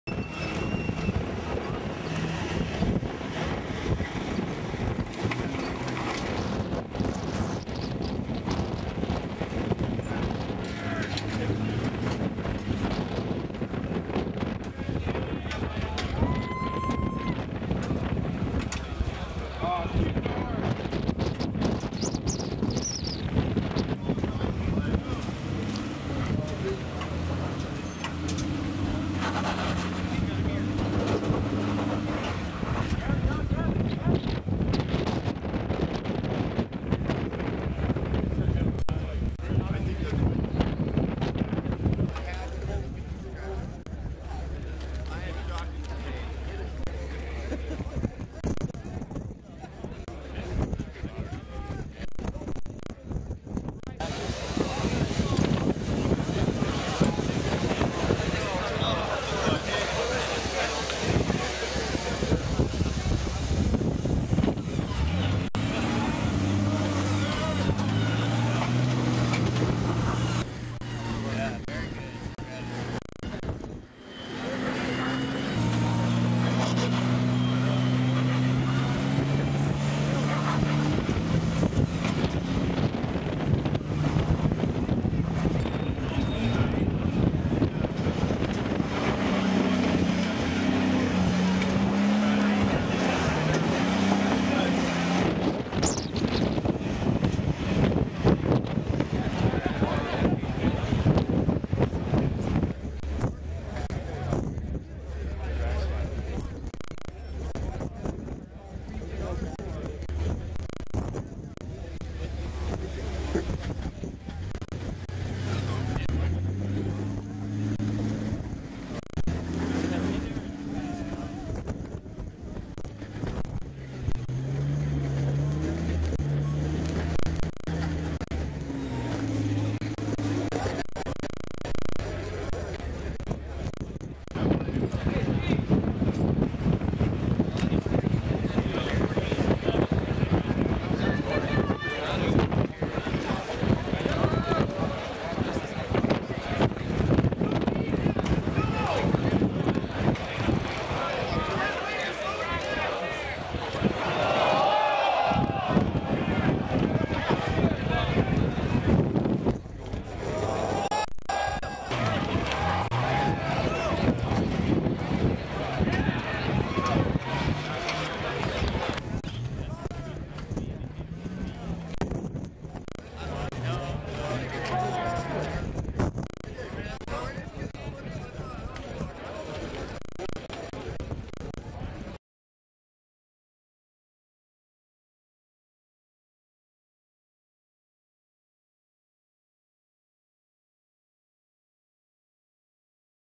Full Body TJ Almost Makes it in LCQ - King of the Hammers February 2009
Kevin #354 takes his full body TJ up Back Door in the Last Chance Qualifier for King of the Hammers.  He only misses it by a couple of minutes.